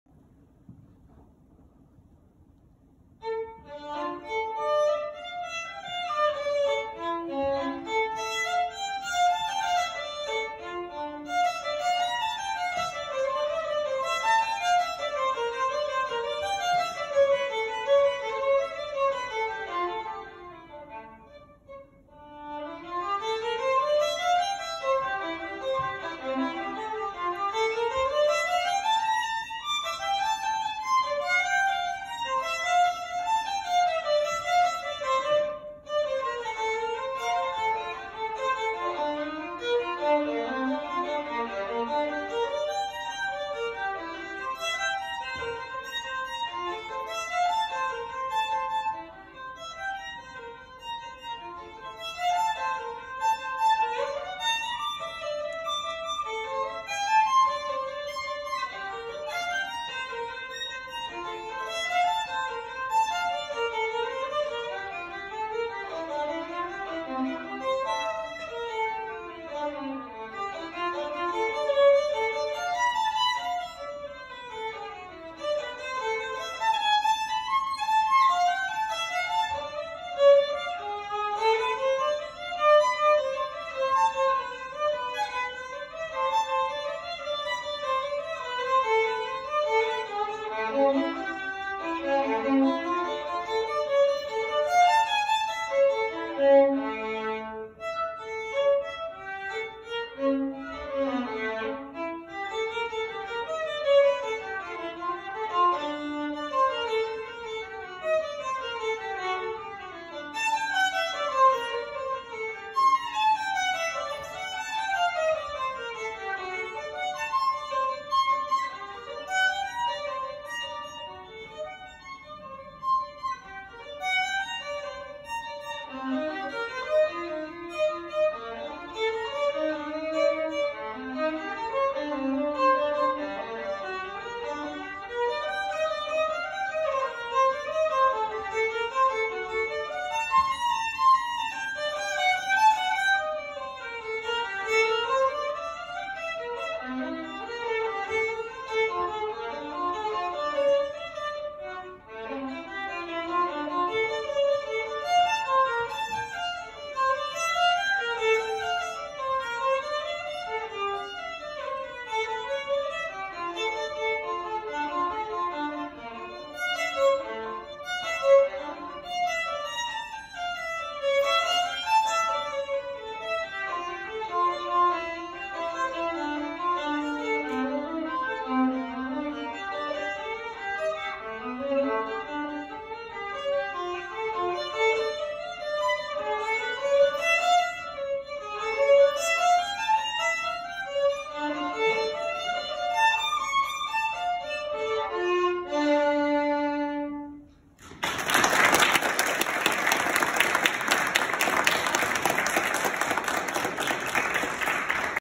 Alexanne Gendron - École secondaire Mont Bleu
Mon concert de violon 2019. Je suis au conservatoire de musique de Gatineau et c’était mon spectacle de Noël donc je voulais participer au concours.